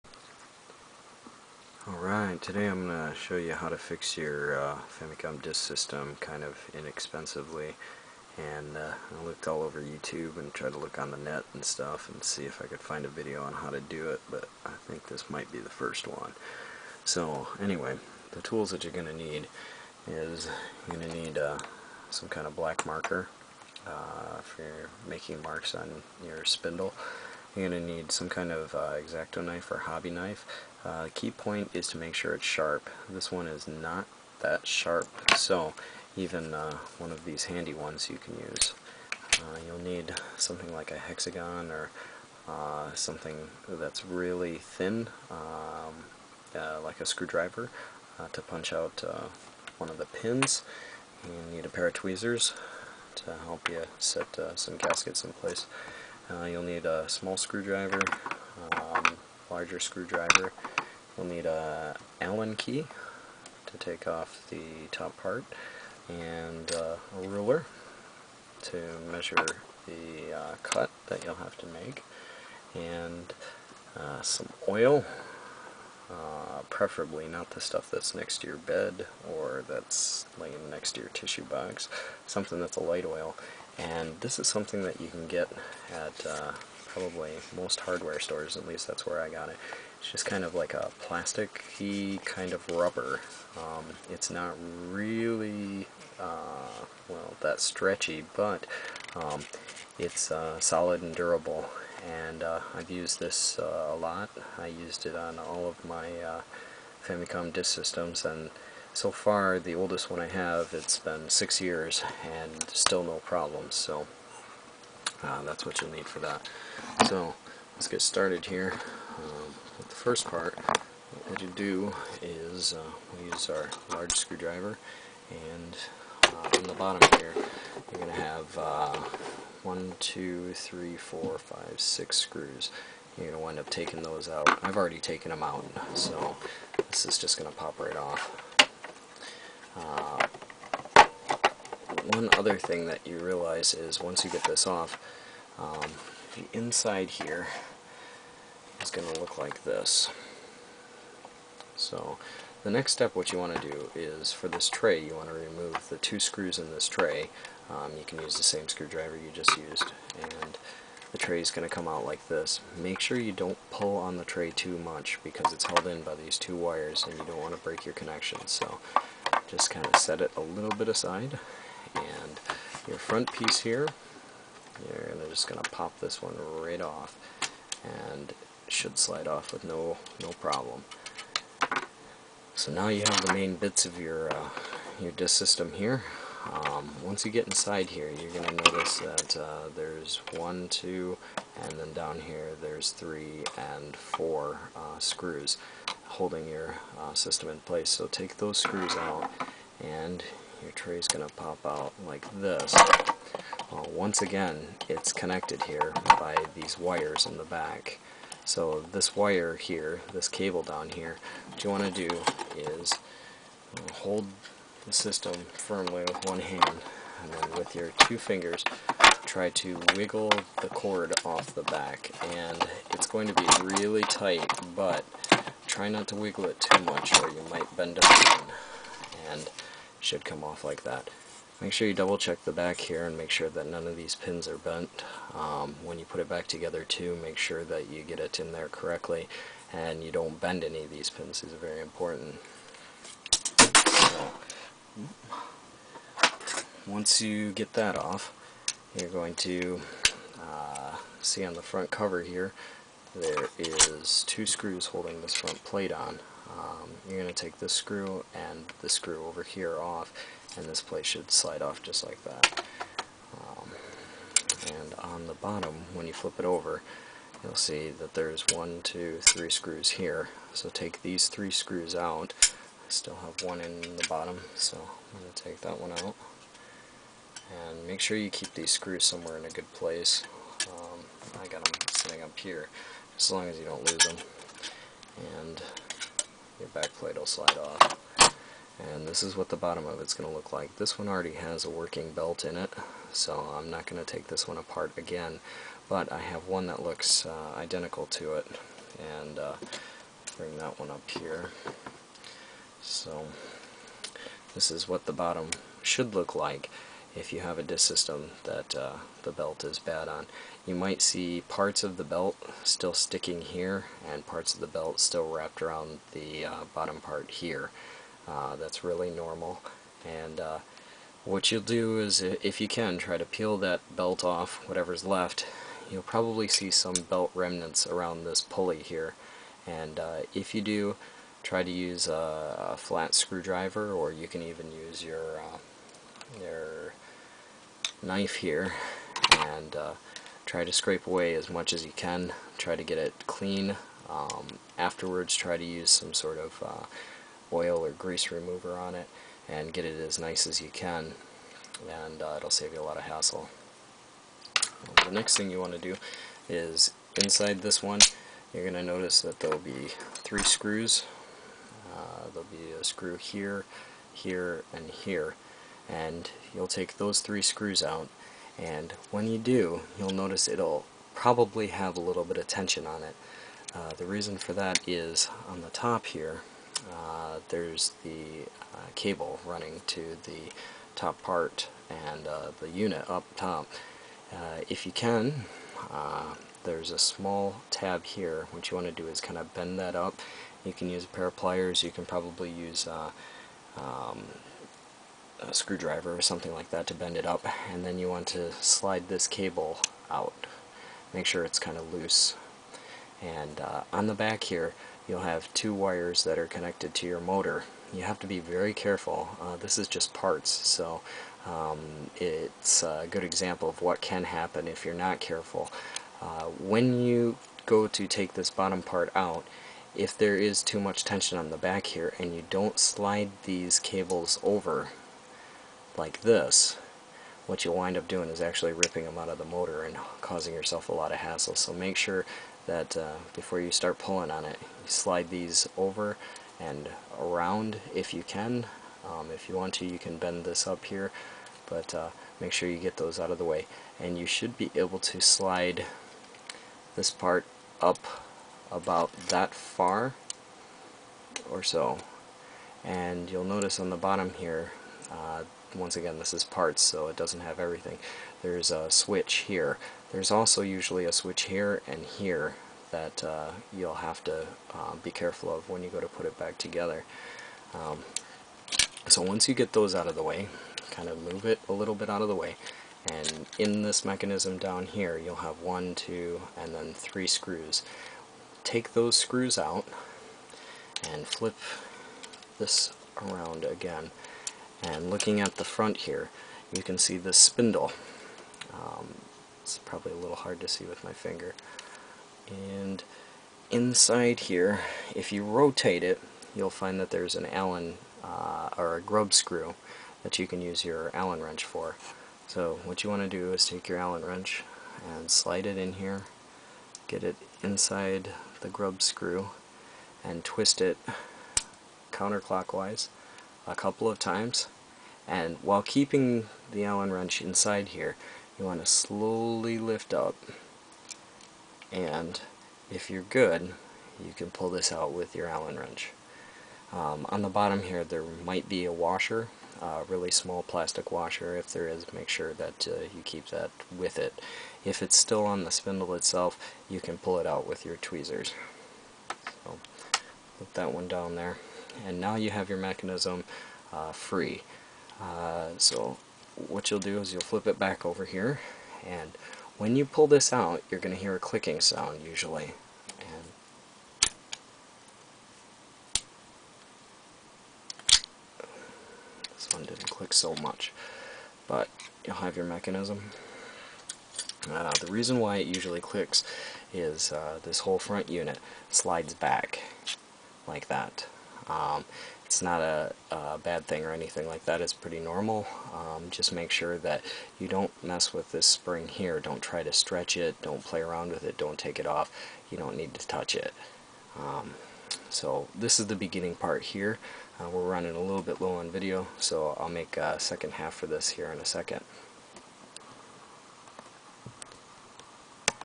How to repair a Famicom Disk System (Part 1)
Part one of a 3 part series.  How to repair the belt on your famicom using things you can get from your hardware store